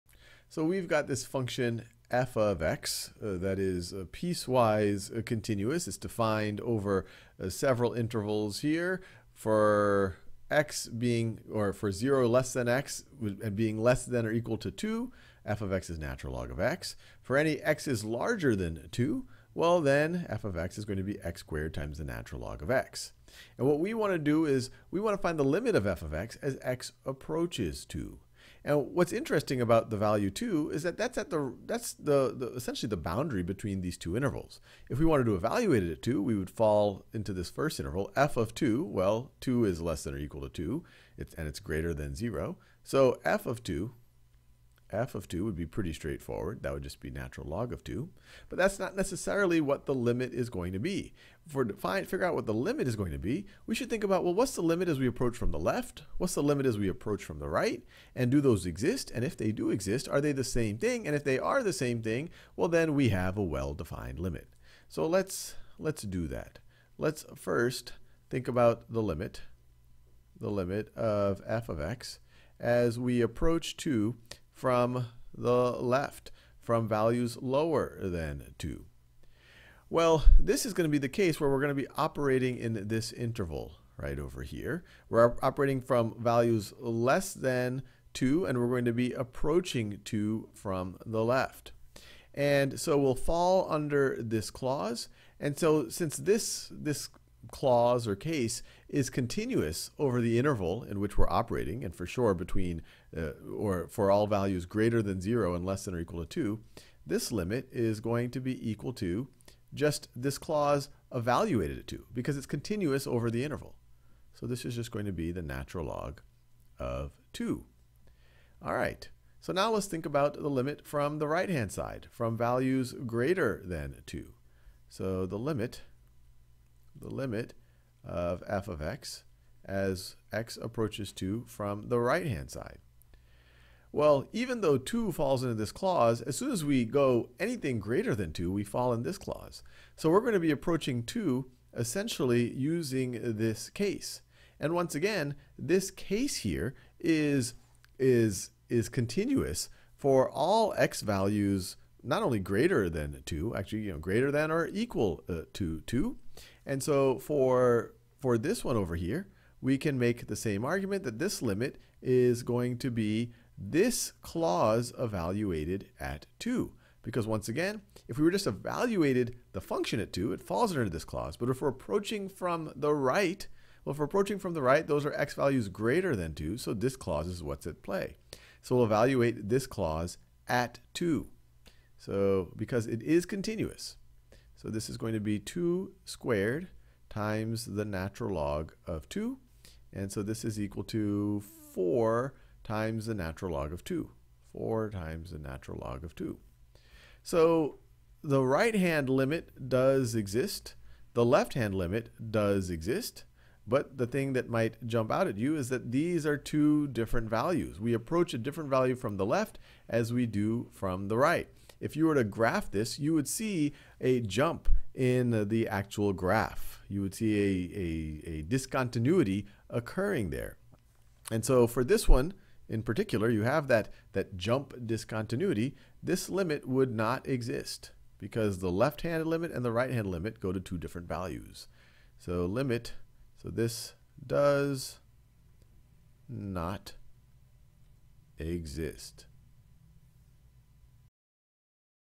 Analyzing functions for discontinuities (discontinuity example) | AP Calculus AB | Khan Academy
Courses on Khan Academy are always 100% free. Start practicing—and saving your progress—now

Sal finds the limit of a piecewise function at the point between two different cases of the function. In this case, the two one-sided limits aren't equal, so the limit doesn't exist.